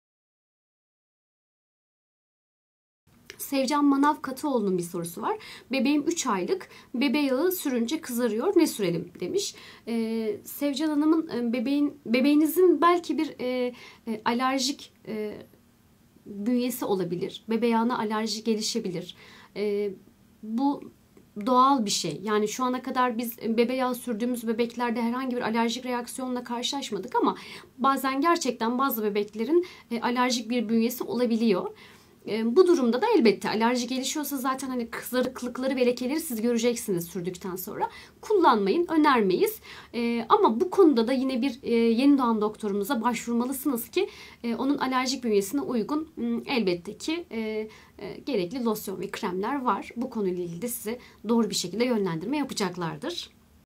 3 Aylık Bebeğime Bebe Yağı Alerji Yapıyor Ne Kullanabilirim ?
Facebook üzerinden yaptığımız canlı yayında YeniDoğan Hemşiresi Sevda TOPAL sorularınızı yanıtladı. İşte Yenidoğan Bakımı ile ilgili en çok merak edilenler!